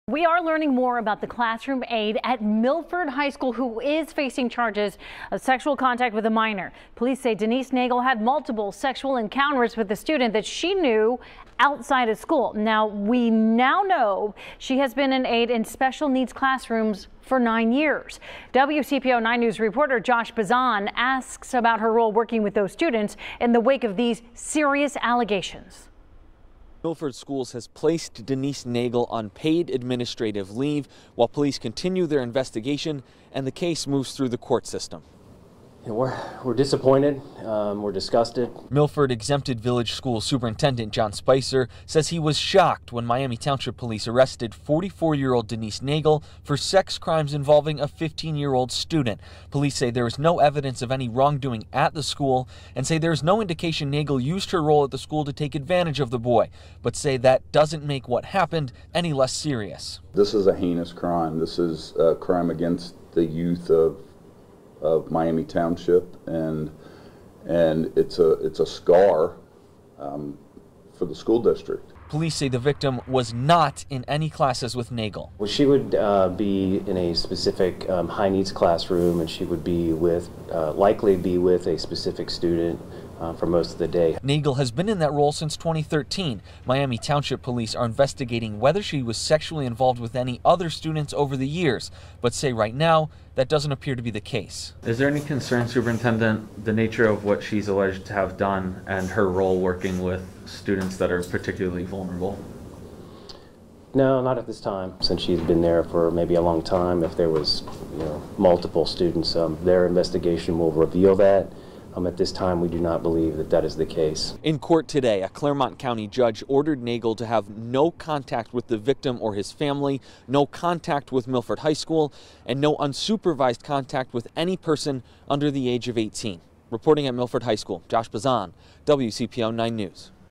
Milford High School superintendent ‘disgusted’ after aide charged with sex crimes involving student
Denise Nagle, 44, stands charged with sexual battery and unlawful sexual contact with a minor.